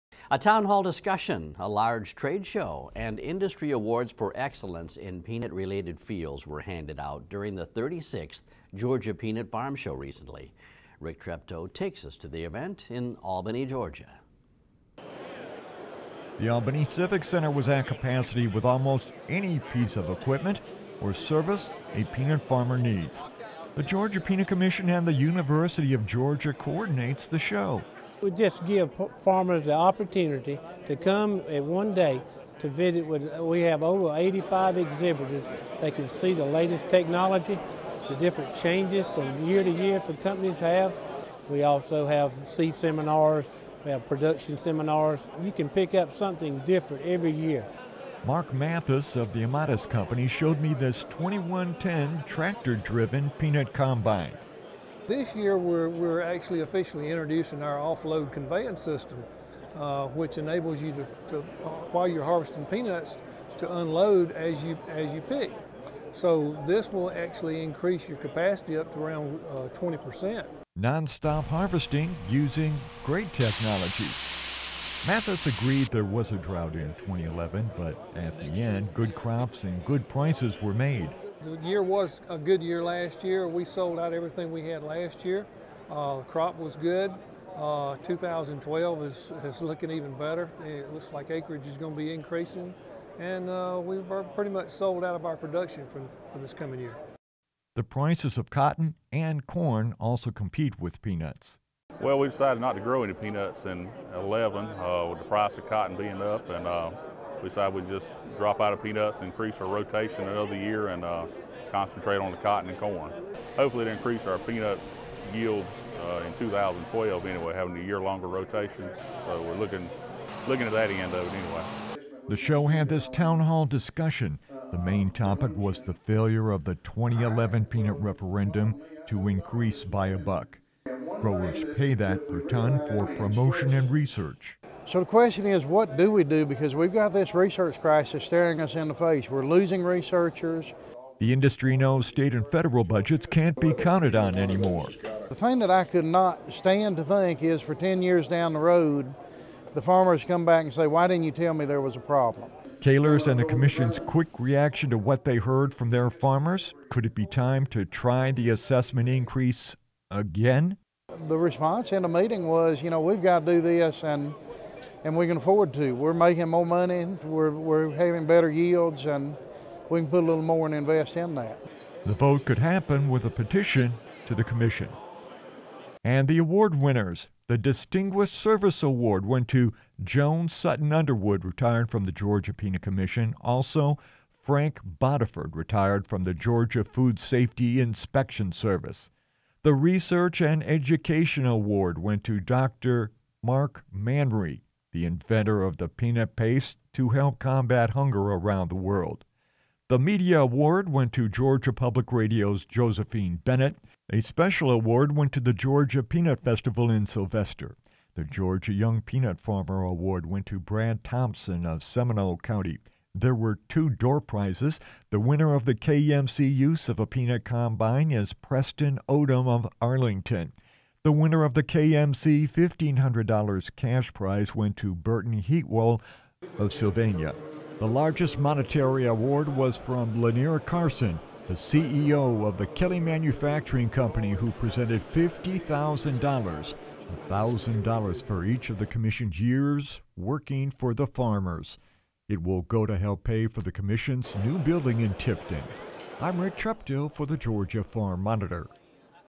Georgia Peanut Farm Show Kicks Off 2012 For Industry
A Town Hall discussion, a large trade show and industry awards for excellence were all part of the recent 36th Georgia Peanut Farm Show.  Rick Treptow takes us to the event in Albany, Georgia.